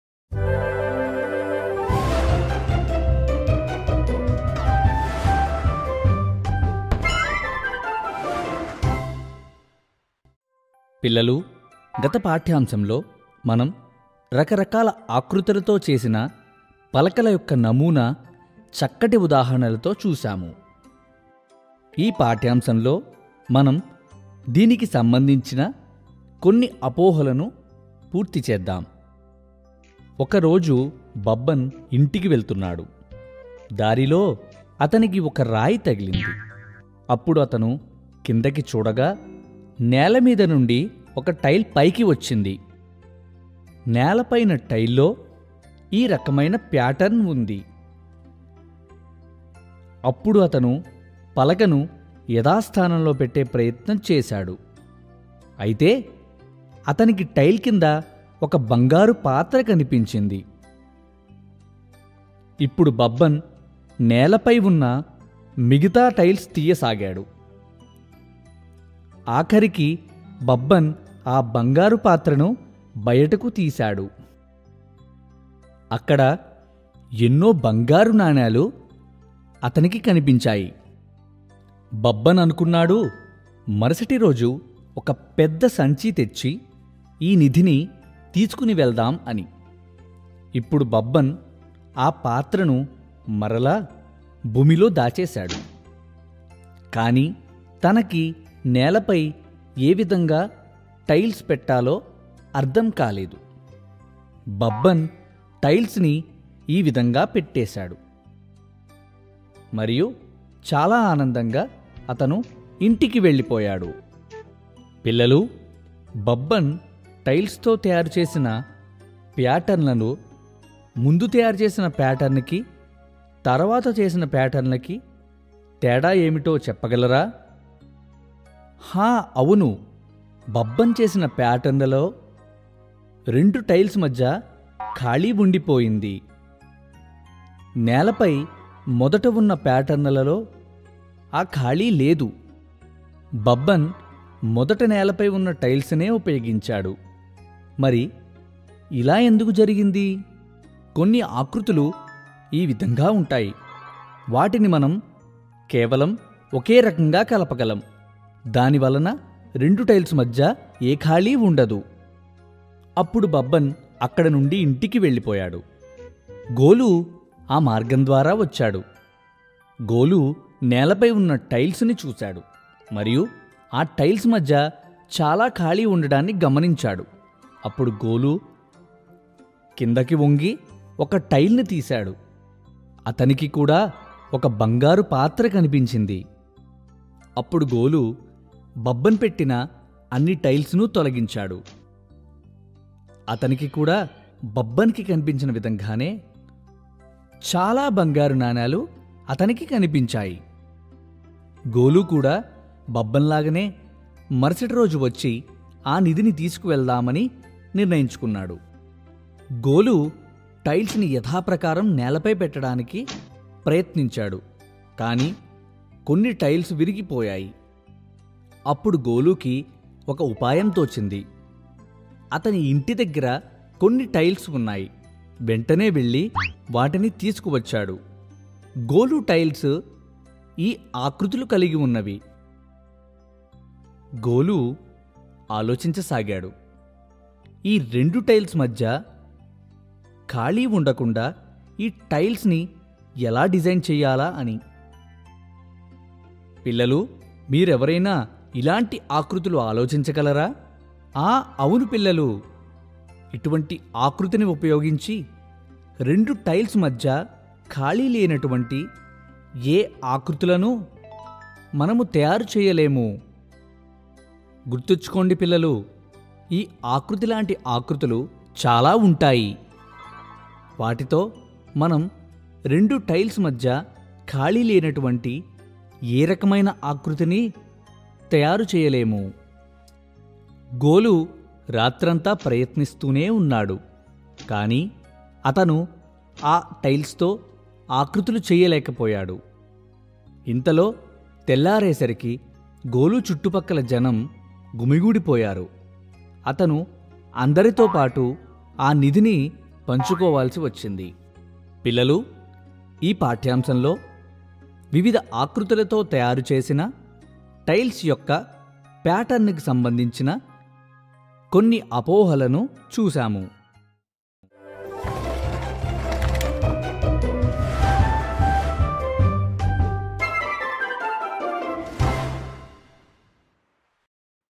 టైల్స్|Part 3/3|Tiling|Telugu|Class 3
"టైల్స్|Part 3/3|Tiling|Telugu|Class 3 | ఆకృతులు మరియు స్థానం | कक्षा 3 | NCERT | ఆకృతుల అద్భుతాలు

Number Patterns:Addition & subtraction | Shapes and Space | Grade 3 | NCERT | Shapes and Design

Welcome to part 3 of Tiling. In this video, we will discuss various misconceptions and/or common errors related to Tiling.